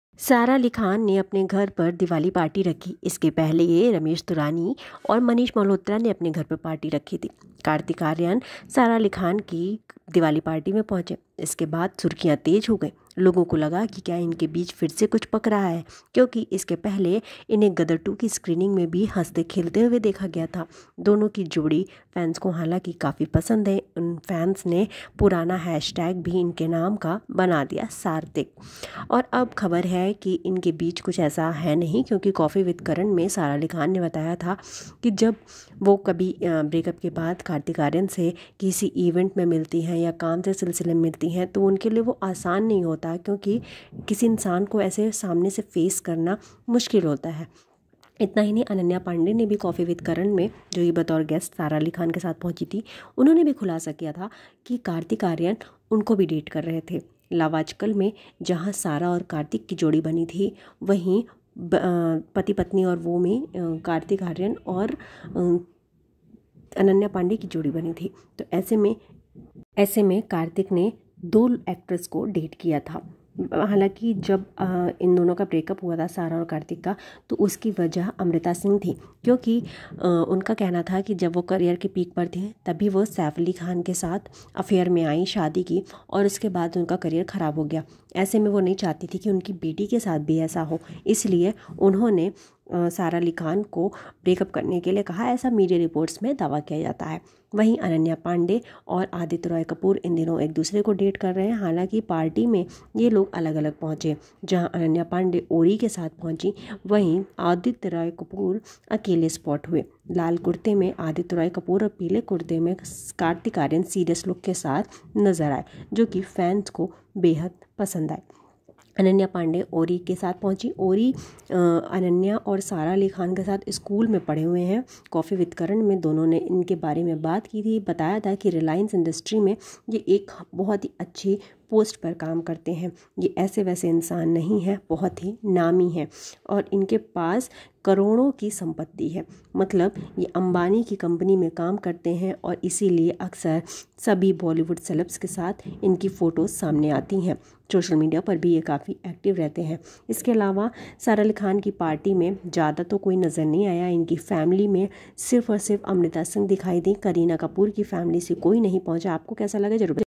अमृता सिंह के सामने करीना कपूर ने कटाई सैफ अली की नाक, सौतेले बच्चों के सामने हुई थू थू
सारा अली खान की सौतेली मां ने बिगाड़ी उनकी पार्टी, सैफ अली खान ने भी किया ड्रामा